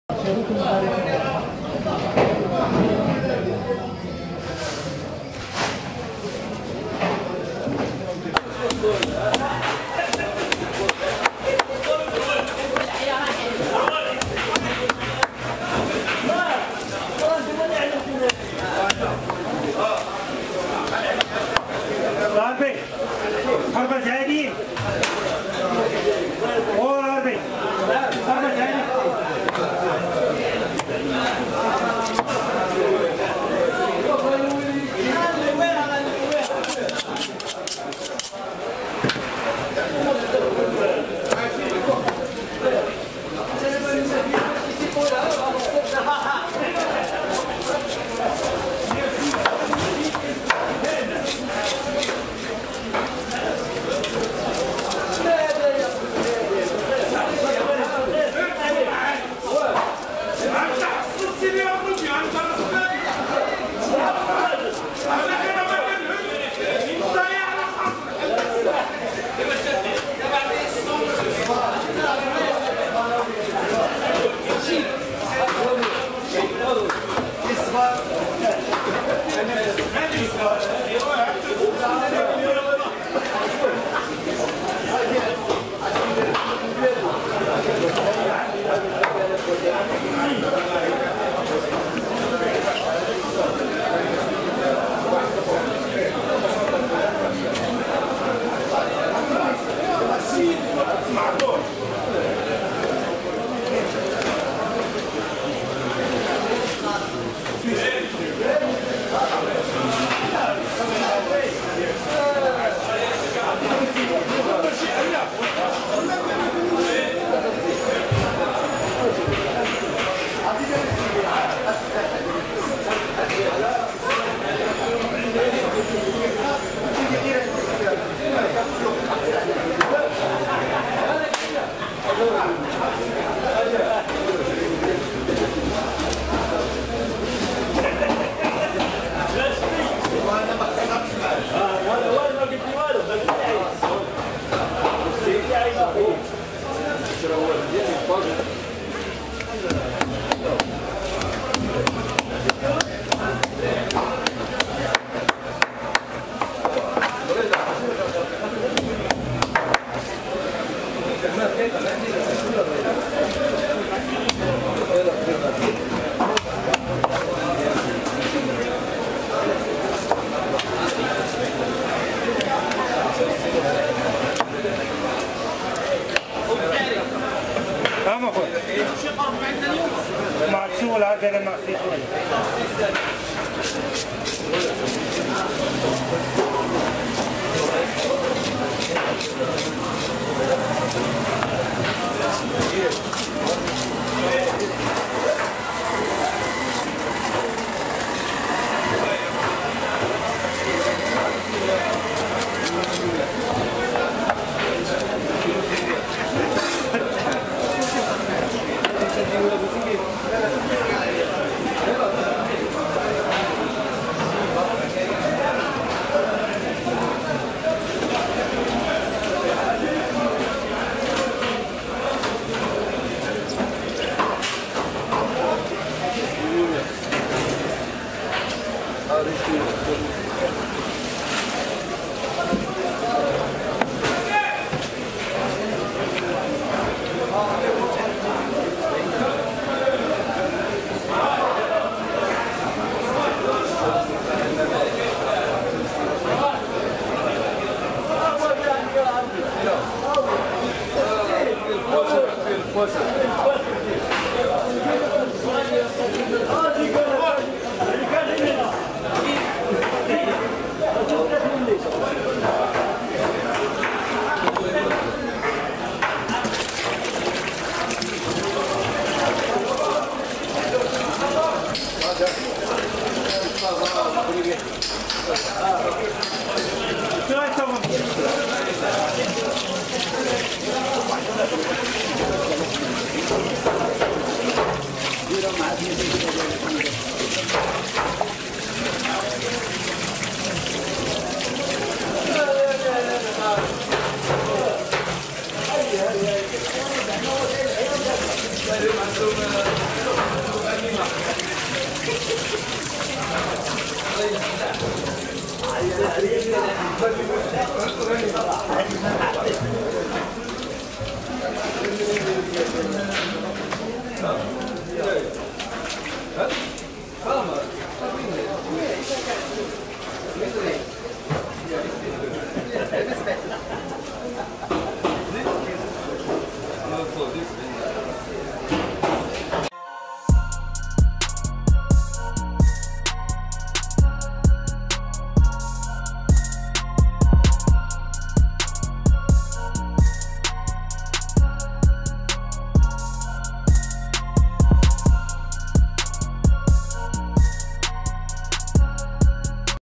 Scaling & Cleaning Big FISH | Best MEAGRE Fish Cleaning and Cutting Skills
How to scale fish very fast has been a skill on Morigami channel !
We are here to make you happy and share with you all Seafood videos !

Meagre (gurbell) is one of the most delicious fish that is available locally. It has a fleshy texture, with a mild taste that will please both fish lovers and those who are not regular fish eaters. When cooked, it almost melts in your mouth. Surprisingly, chicken stock is a great base for cooking meagre. The fish overpowers the taste of the chicken stock, to produce very delicious flavours.

Another very quick way to cook filleted meagre is to steam it in an infusion of chicken stock for around 12 minutes, depending on the thickness of the fillet. Allow 150g to 200g of filleted meagre per portion.

Relatively big head with elongated body. Mouth in terminal position without barbils. Eyes quite small. Lateral line evident, extending onto caudal fin. Second dorsal fin much longer than first. Anal fin has a first short spiny ray and a second very thin one. Several branched appendices are present in the gas bladder, which can vibrate producing a typical 'grunt'. Very large otoliths. Body colour silver-grey, with bronze traits dorsally. Fin base reddish brown and mouth cavity yellow-gold. Post-mortem colour brown. Reaches up to 2 m in length and 50 kg in weight.